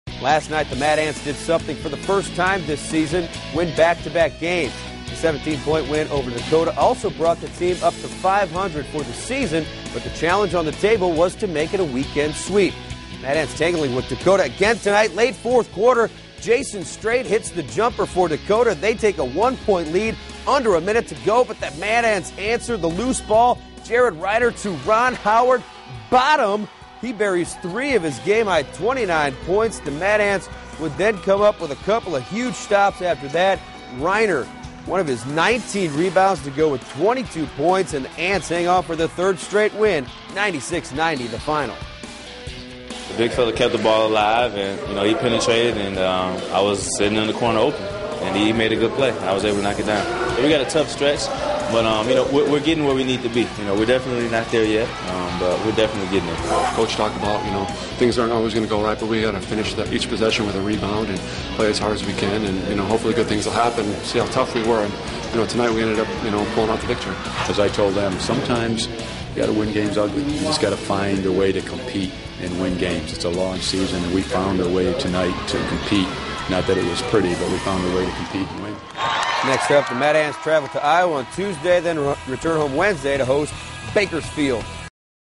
Mad Ants win third straight topping Dakota Sunday
Ron Howard drained a three-pointer from the corner with under a minute left to give the Mad Ants the lead for good as they held off Dakota for the win Sunday, 96-90.  Newly acquired Jared Reiner had 22 points and 19 rebounds.